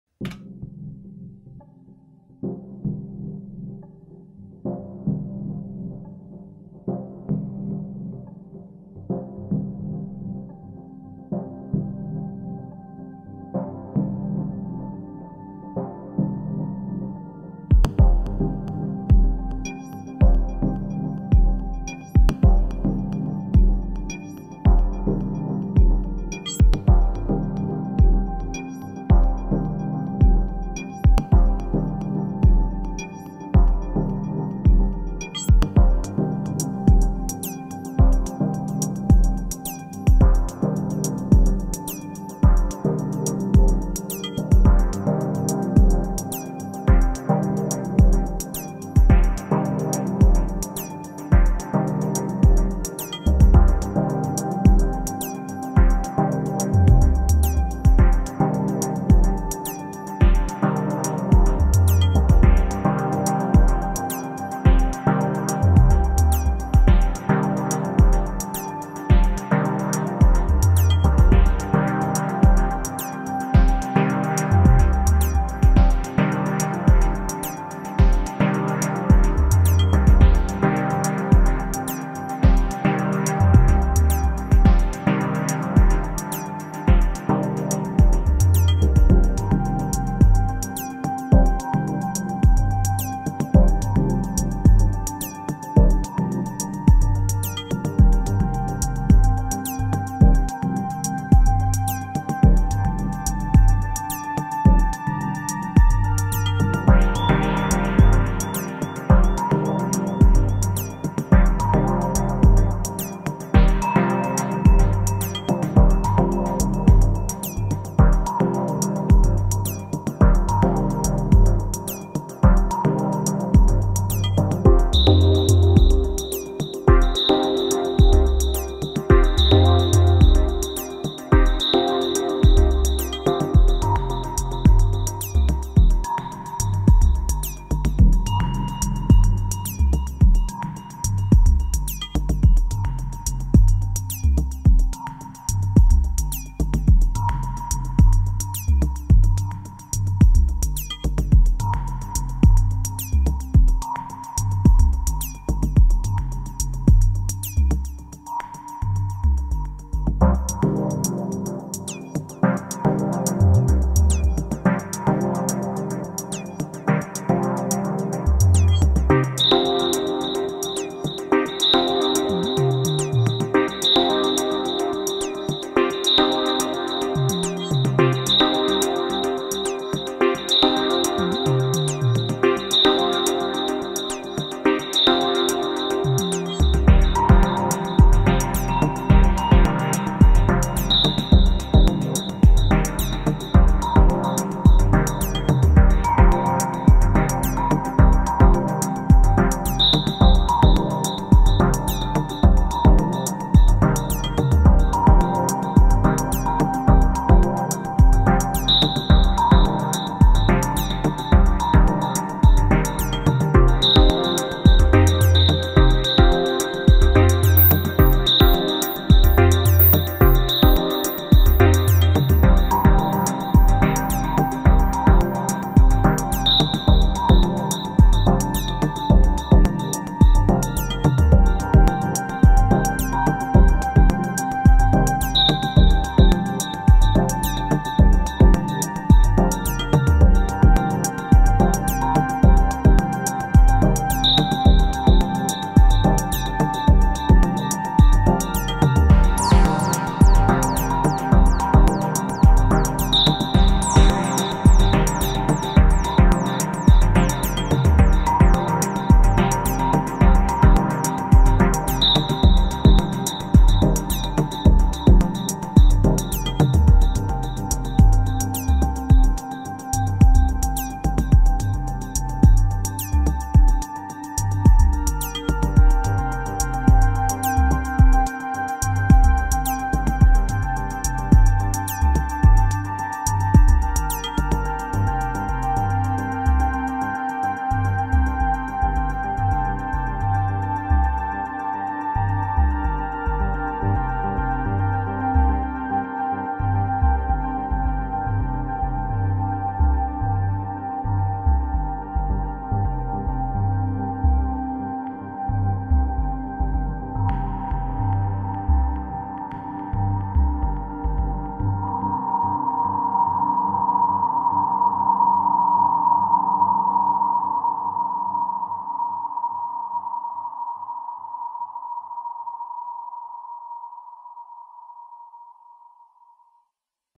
Analog Four Live Session - Dub/Ambient - Undvlpt
Live session using only the four tracks of the Elektron Analog Four. Quite an amazing little machine that you can get way more than four tracks out of with the trigs feature.
Hope you enjjoyyyyyy. Thanks for stopping by!

Undvlpt.